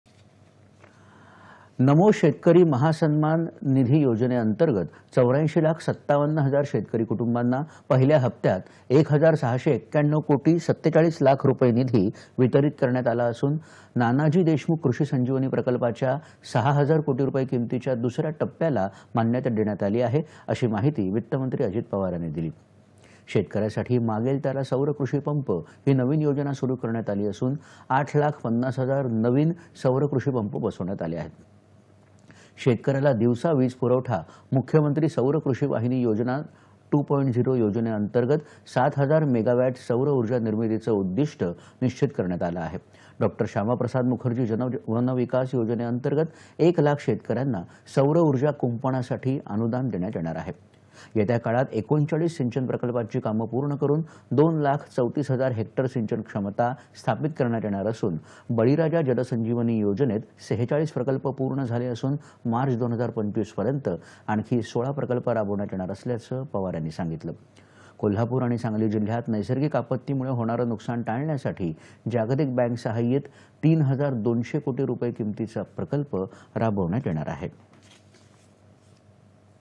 अर्थसंकल्‍प शेती : शेतकऱ्यांसाठी मागेल त्याला सौर कृषि पंप ही नवीन योजना सुरु करण्यात आली
दि. २७.०२.२०२४ :
  नमो शेतकरी महासन्मान निधी योजनेअंर्तगत ८४ लाख ५७ हजार शेतकरी कुटुंबांना पहिल्या हप्त्यात १ हजार ६९१ कोटी ४७ लाख रुपये निधी वितरित करण्यात आला असून नानाजी देशमुख कृषि संजीवनी प्रकल्पाच्या ६ हजार कोटी रुपये किमतीच्या दुसऱ्या टप्प्याला मान्यता देण्यात आली आहे, अशी माहिती वित्तमंत्री अजित पवार यांनी दिली.
        शेतकऱ्यांसाठी मागेल त्याला सौर कृषि पंप ही नवीन योजना सुरु करण्यात आली असून ८ लाख ५० हजार नवीन सौरकृषी पंप बसवण्यात आले आहेत.
      शेतकऱ्याला दिवसा वीज पुरवठा -मुख्यमंत्री सौर कृषी वाहिनी योजना 2.0 योजनेअंतर्गत ७ हजार मेगावॅट सौर ऊर्जा निर्मितीचे उद्दिष्ट निश्चित करण्यात आलं आहे. डॉ. श्यामाप्रसाद मुखर्जी जन वनविकास योजनेअंतर्गत १ लाख शेतकऱ्यांना सौर उर्जा कुंपणासाठी अनुदान देण्यात येणार आहे.
     येत्या काळात ३९ सिंचन प्रकल्पांची कामं पूर्ण करुन २ लाख ३४ हजार हेक्टर सिंचन क्षमता स्थापित करणार असून बळीराजा जलसंजीवनी योजनेत ४६ प्रकल्प  पूर्ण  झाले असून मार्च २०२५ पर्यंत आणखी 16 प्रकल्प राबविण्यात येणार असल्याचं पवार यांनी सांगितलं.
    कोल्हापूर आणि सांगली जिल्ह्यात नैसर्गिक आपत्तीमुळे होणारे नुकसान टाळण्यासाठी जागतिक बँक सहाय्यित 3 हजार 200 कोटी रुपये किंमतीचा प्रकल्प राबविण्यात येणार आहे.

8.30am | 11.00am | 1.00pm | 4.30pm | 7.00pm | 9.30pm